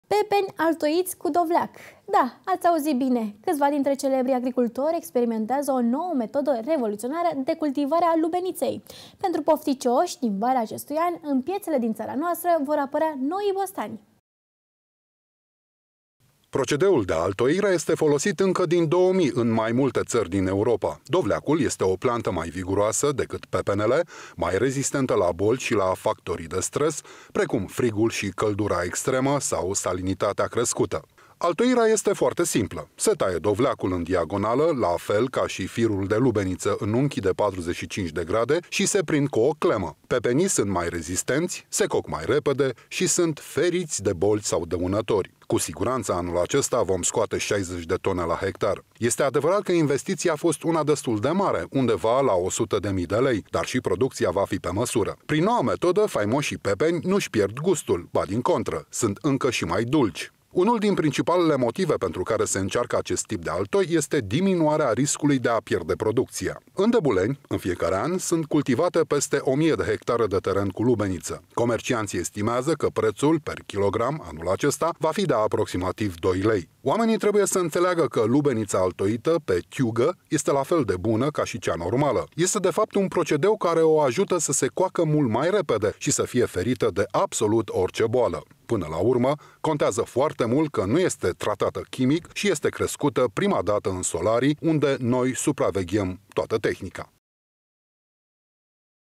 Tehnologie olteneasca! Pepenii se altoiesc cu dovleci la Dabuleni
Pepeni altoiti cu dovleac. Da, ati auzit bine. Cativa din celebrii agricultori din localitate, experimenteaza o noua metoda revolutionara de cultivare a lubenitei. Pentru pofticiosi, din vara acestui an, in pietele din tara noastra vor aparea noii bostani.